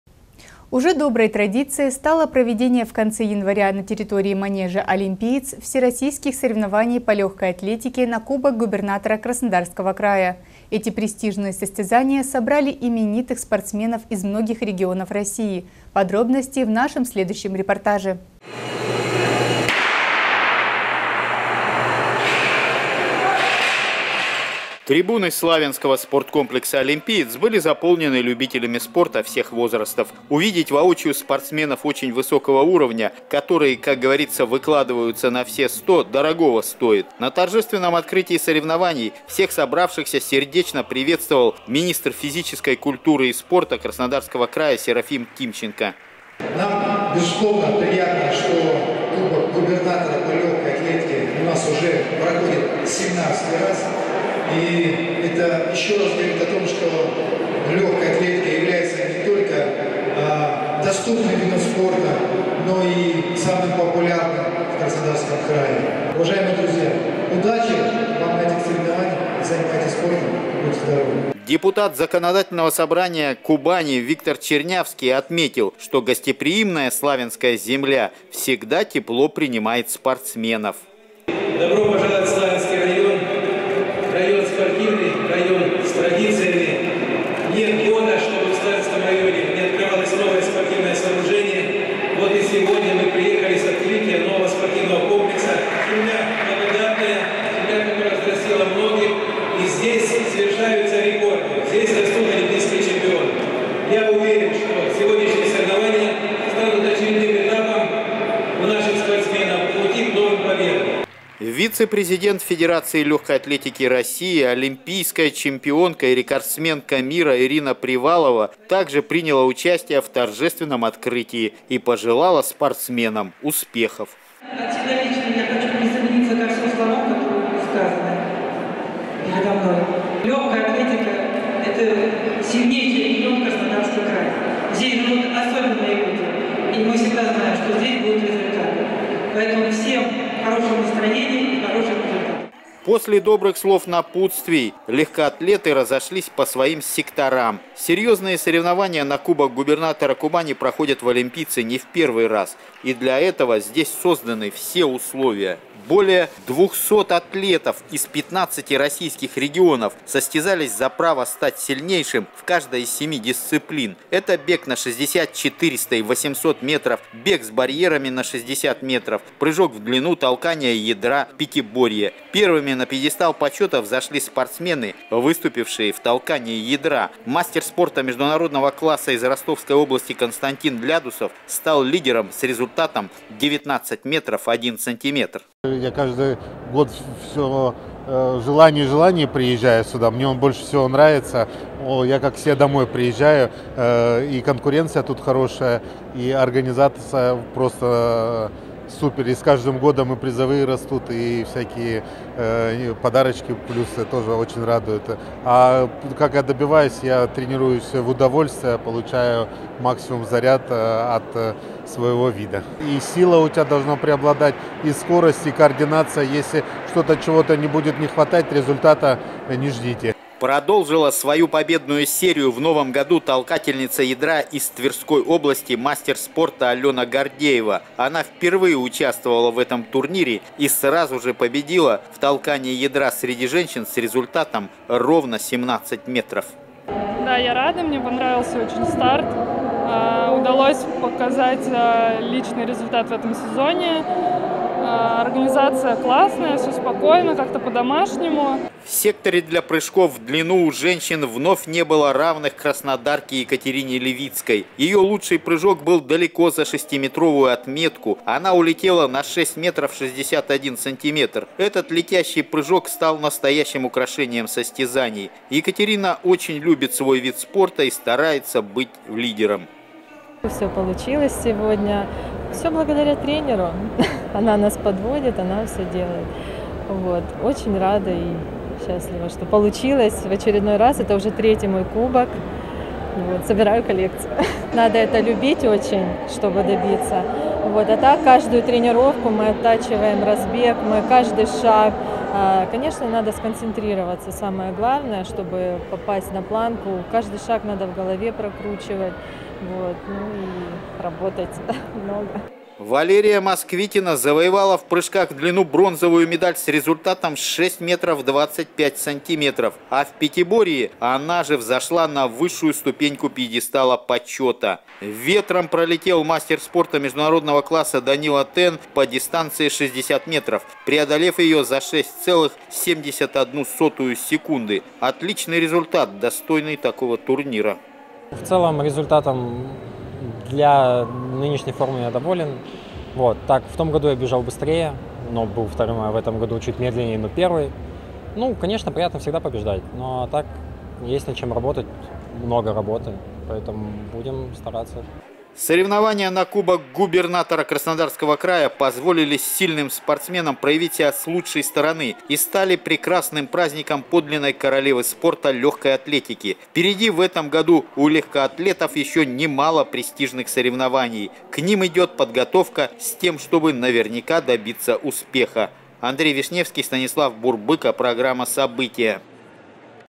Праздник королевы спорта
Уже доброй традицией стало проведение в конце января на территории манежа «Олимпиец» Всероссийских соревнований по легкой атлетике на Кубок губернатора Краснодарского края. Эти престижные состязания собрали именитых спортсменов из многих регионов России. Подробности– в нашем следующем репортаже